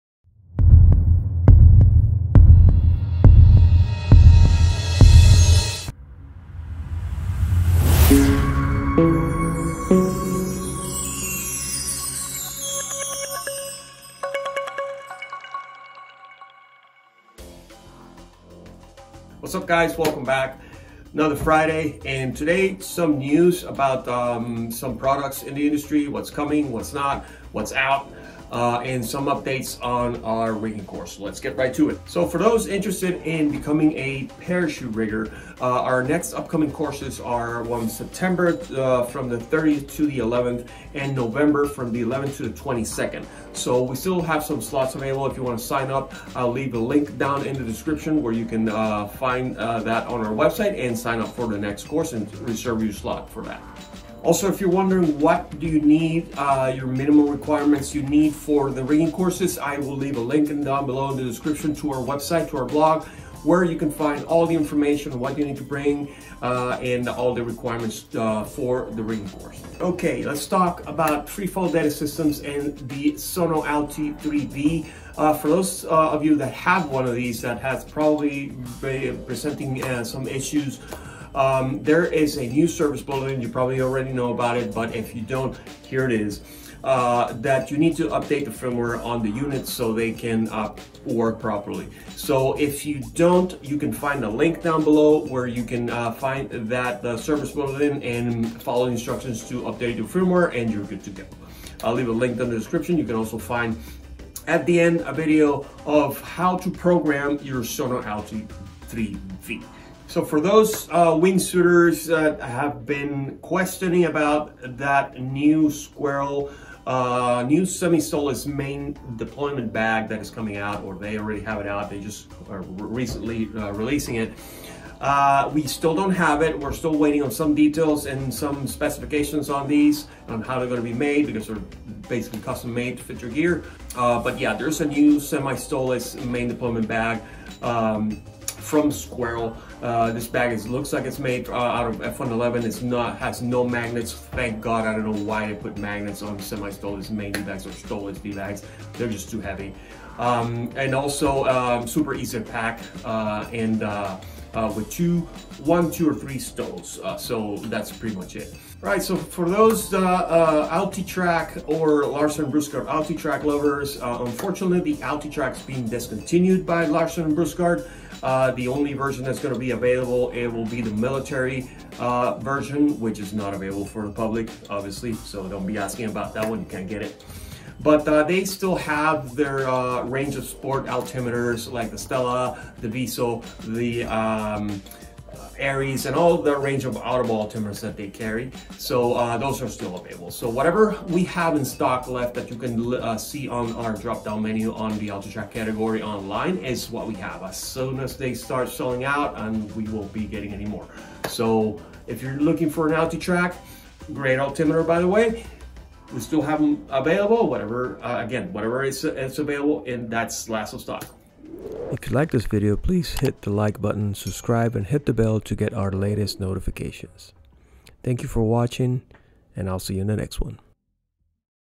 May 2024 News Update
A brief news update of new accessories, discontinued items and upcoming rigging courses.